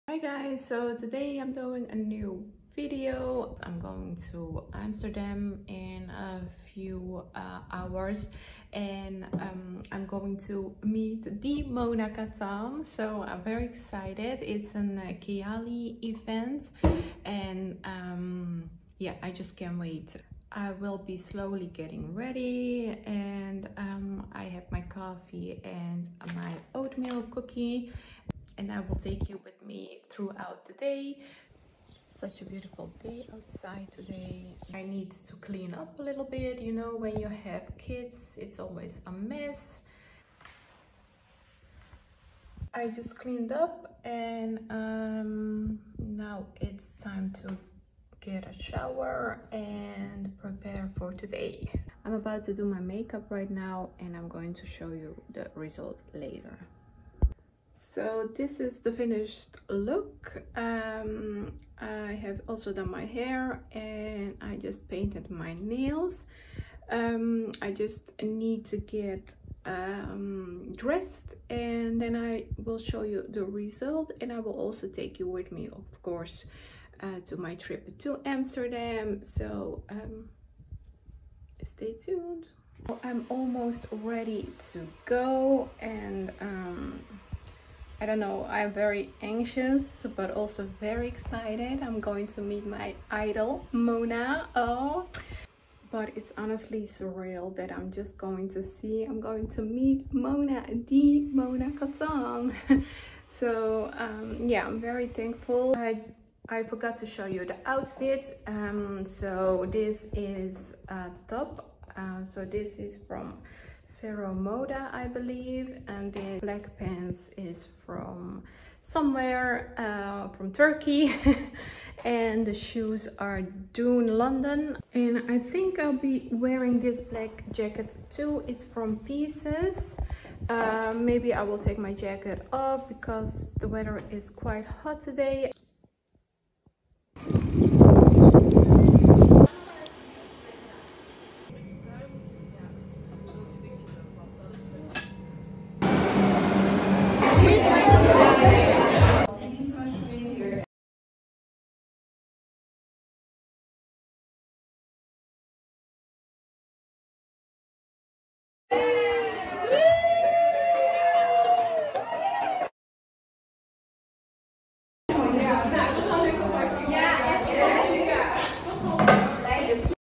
KAYALI Vanilla Candy Rock Sugar event with Mona in AMSTERDAM vlog | SELMA
Hi everyone,

When dream become reality! I met Mona at the Kayali event in Amsterdam. Memories are made!
Thank you Maison PR for making it happen! 💜